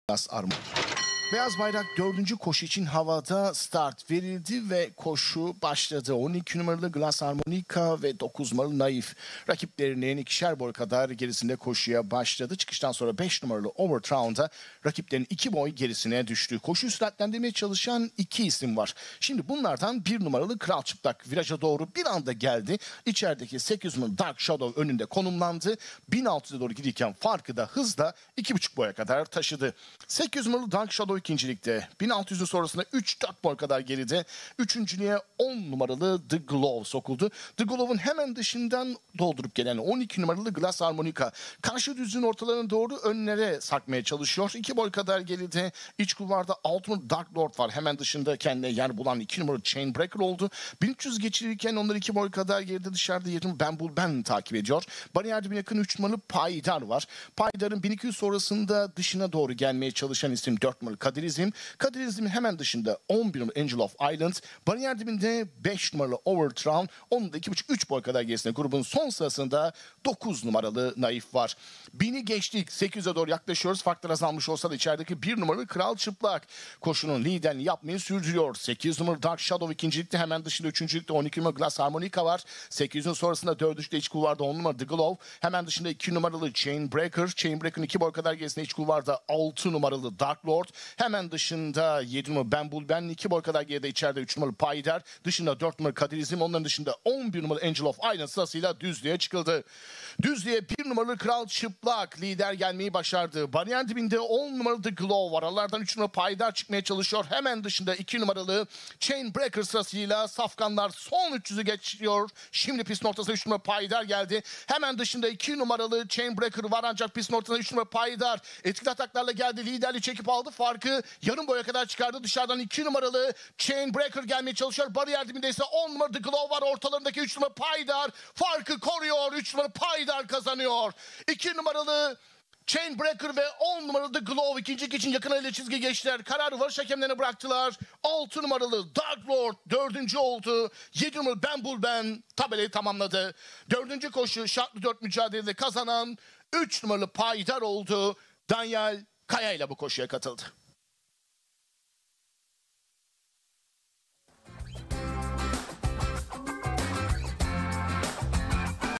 🥇 Payidar by 𝐒𝐌𝐀𝐑𝐓 𝐑𝐎𝐁𝐈𝐍
Şartlı.4
🇹🇷 Istanbul
2100mt Kum

🥇 Payidar
(𝐒𝐌𝐀𝐑𝐓 𝐑𝐎𝐁𝐈𝐍 - Amazing Grace by Always A Classic)

Sahip: Emre Kisbu
Antrenör: Suat Özkan
Jokey: Danyal Kaya
Yetiştirici: Kadriye Meliha Eliyeşil